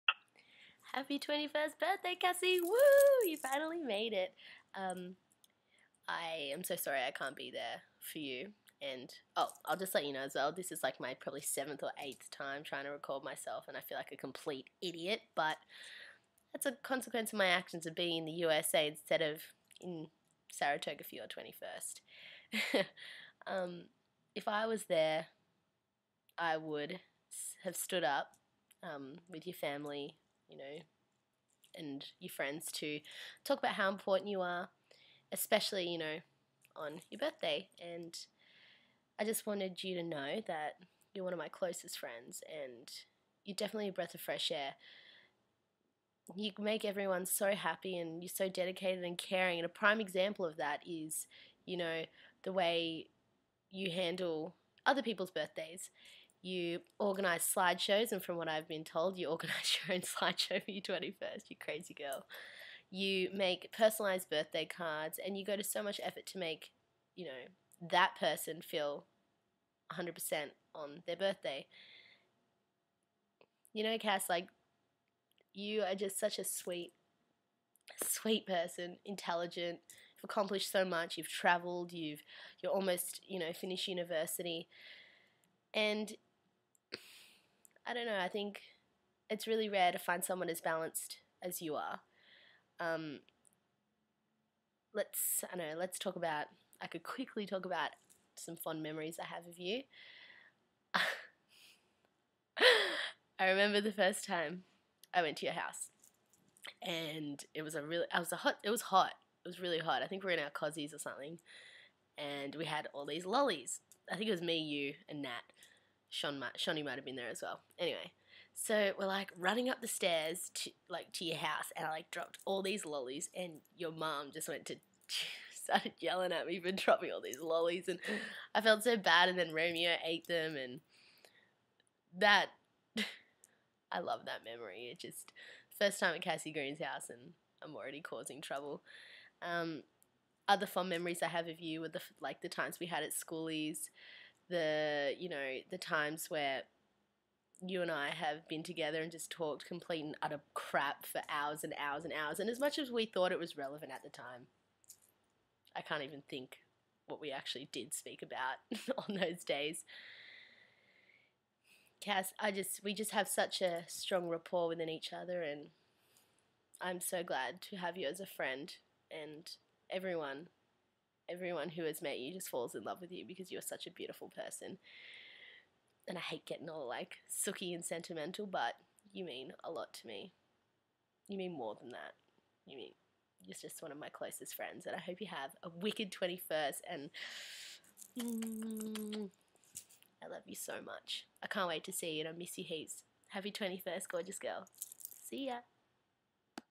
Cassie's 21st Birthday
This is a short birthday message for my wonderful friend, Cassie. I wasn't able to make it to her 21st birthday party in Australia because I am living in the US. Happy birthday, Chicken Nugget!